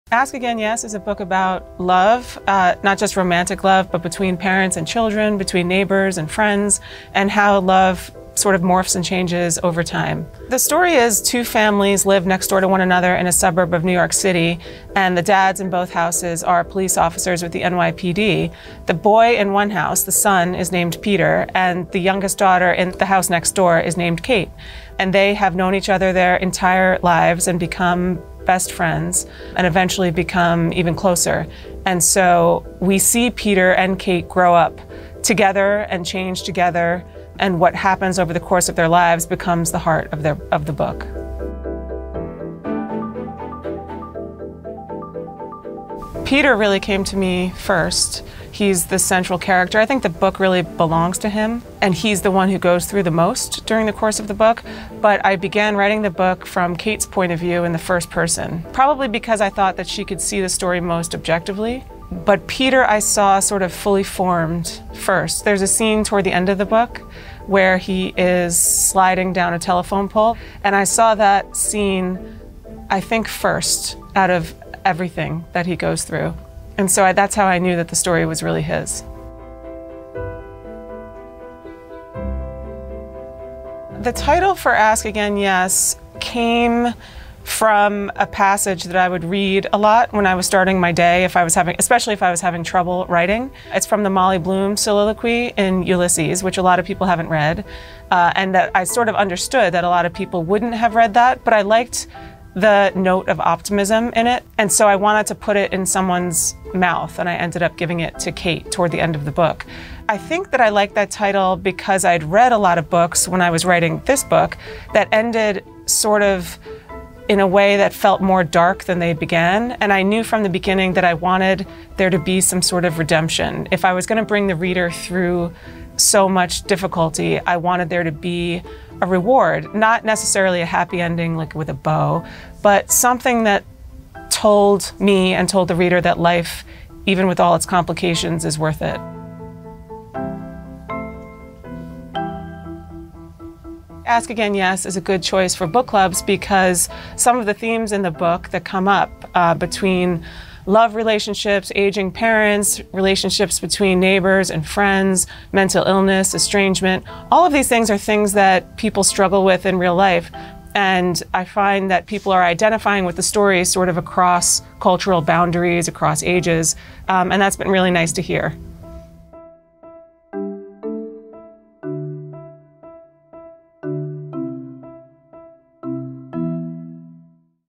How Stories of Redemption Influenced Mary Beth Keane's Ask Again, Yes
New York Times bestselling author of ASK AGAIN, YES, Mary Beth Keane, discusses the first scene that she imagined for the book, the passage from James Joyce's Ulysses that inspired the title, and more.